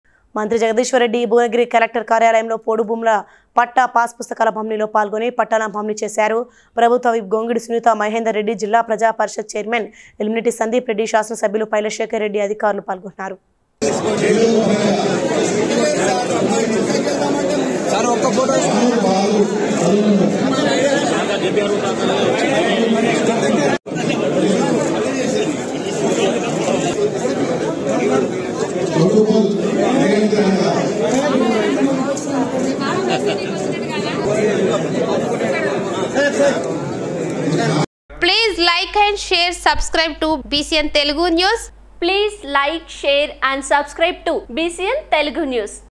పోడుభూముల పట్టా పాస్ పుస్తకాల పంపిణీలో పాల్గొన్న: ప్రభుత్వ VIP సునీత మహేందర్ రెడ్డి
మంత్రి జగదీశ్వర్ రెడ్డి భువనగిరి కలెక్టర్ కార్యా లయంలో పోడుభూము ల పట్టా పాస్ పుస్తకాల పంపిణీలో పాల్గొని,పట్టాలను పంపిణి చేశారు. ప్రభుత్వ విప్ గొంగిడి సునీత మహేందర్ రెడ్డి, జిల్లా ప్రజా పరిషత్ చైర్మన్ ఎలిమినేటి సందీప్ రెడ్డి, శాసనసభ్యులు పైళ్ల శేఖర్ రెడ్డి,అధికారు లు పాల్గొన్నారు.